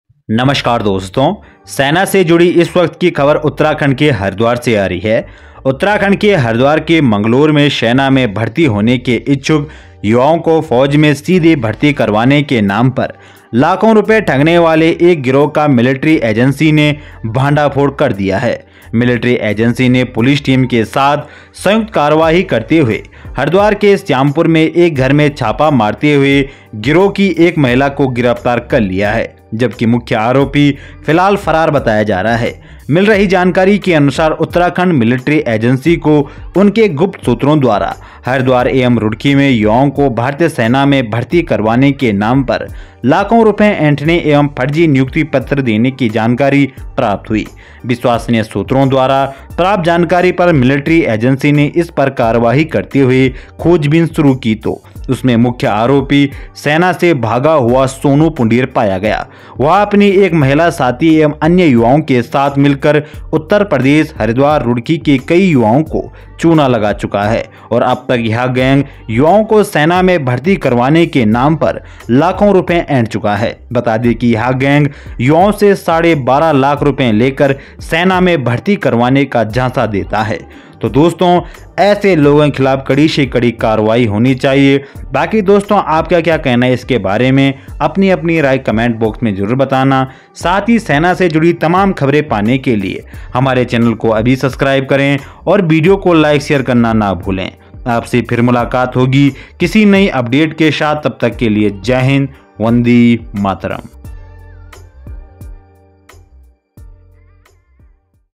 सेना के भगौड़े सोनू ने गर्लफ्रेंड के साथ मिलकर किया कांड || युवाओं से लाखों की लूट
हरिद्वार के मंगलौर में सेना में भर्ती होने के इच्छुक युवाओं को फौज में सीधे भर्ती करवाने के नाम पर लाखों रुपए ठगने वाले एक गिरोह का मिलिट्री इंटेलिजेंस ने भंडाफोड़ कर दिया है।।।